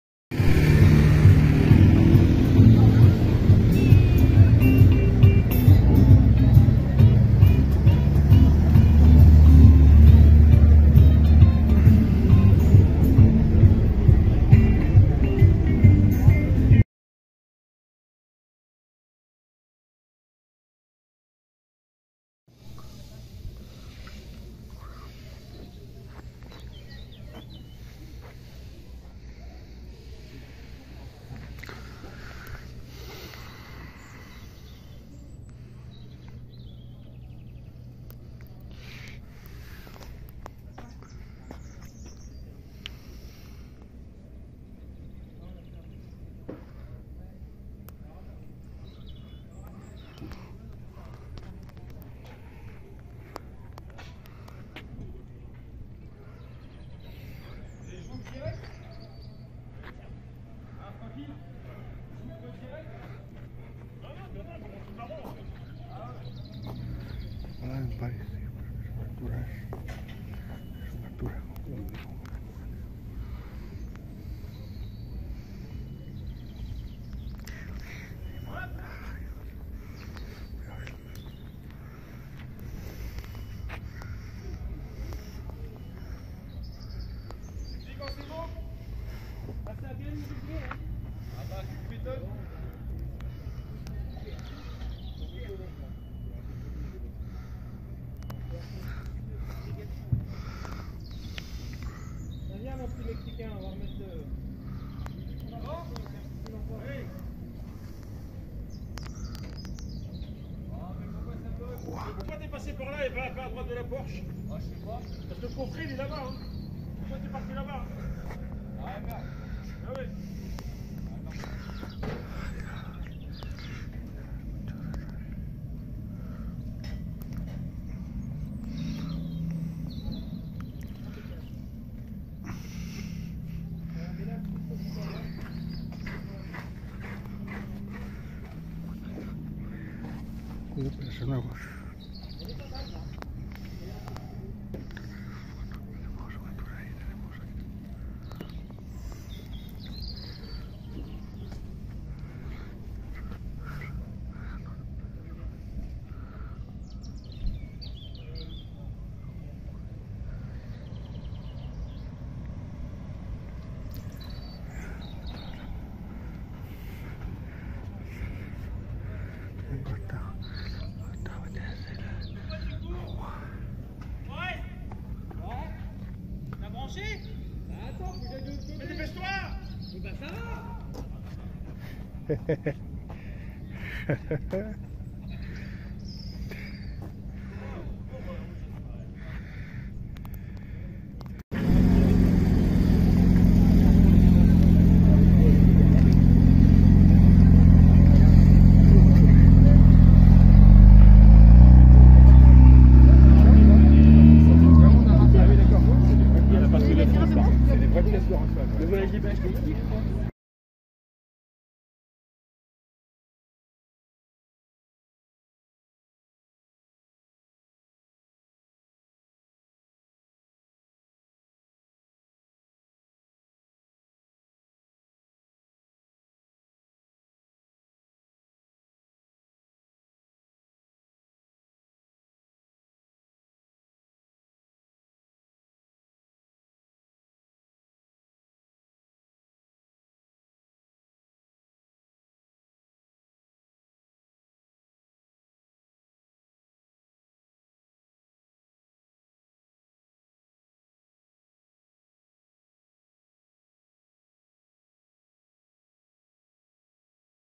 mclaren artura syc 2023
EN ESTE NUEVO VIDEO ENSEÑAMOS EL MCLAREN ARTURA,VISTO EN KDD DEPORTIVOS EN FRANCIA